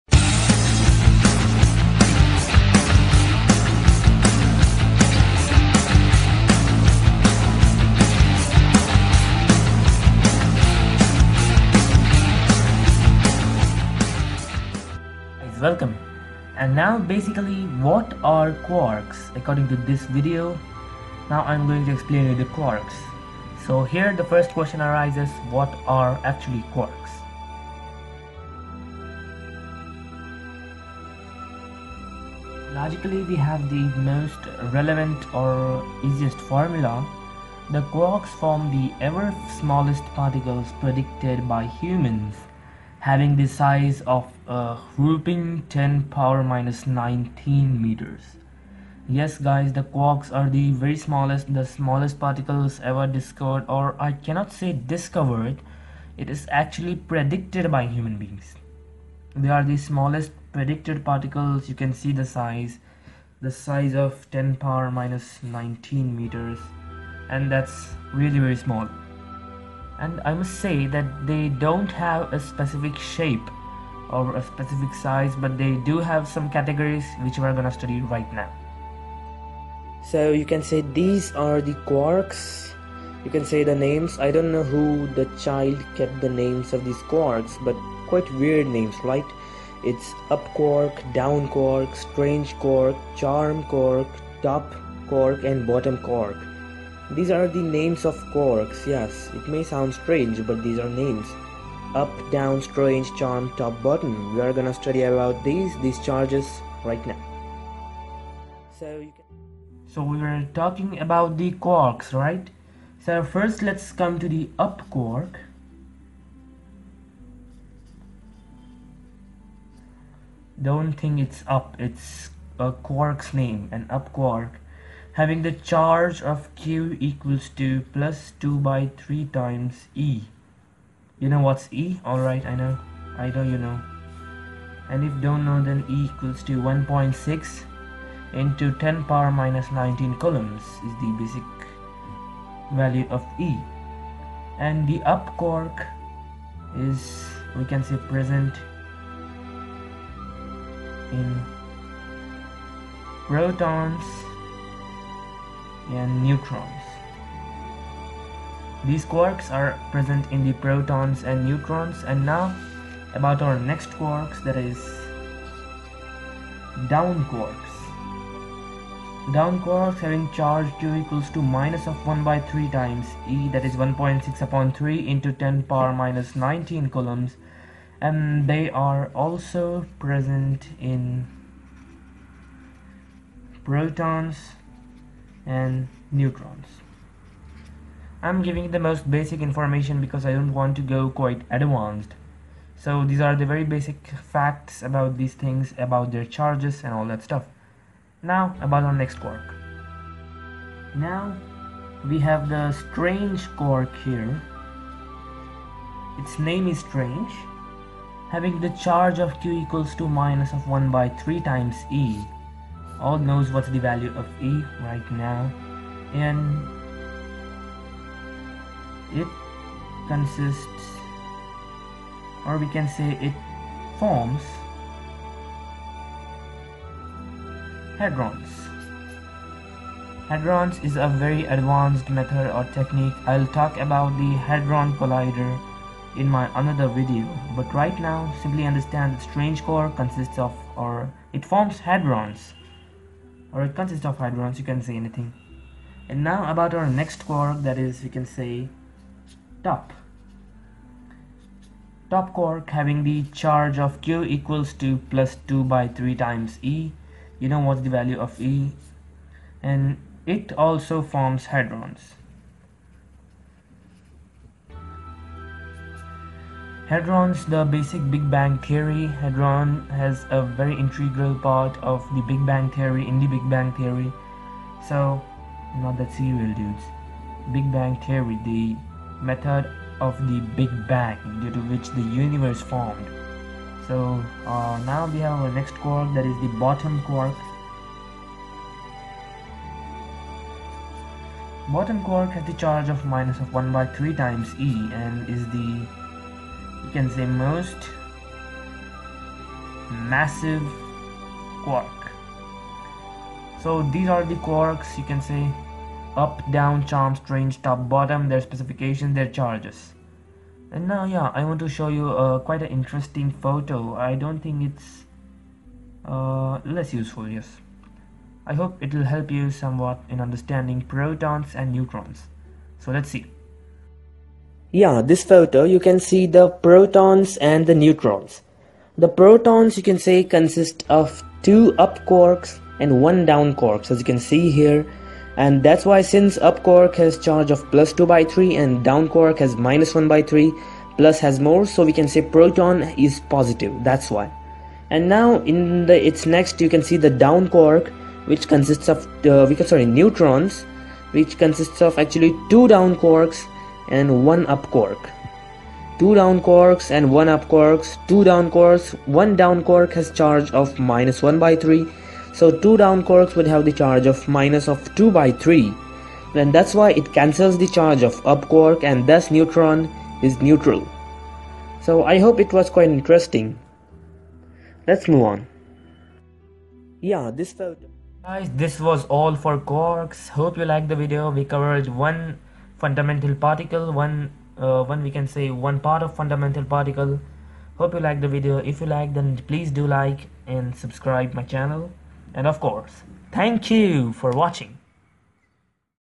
"Quarks" ... in some 6 minutes
Hey viewers...
In this video I am explaining about the Quarks. its charges and it's types
Some strange wierd names of quarks...
And yeah. the have size of .10^-19 metres.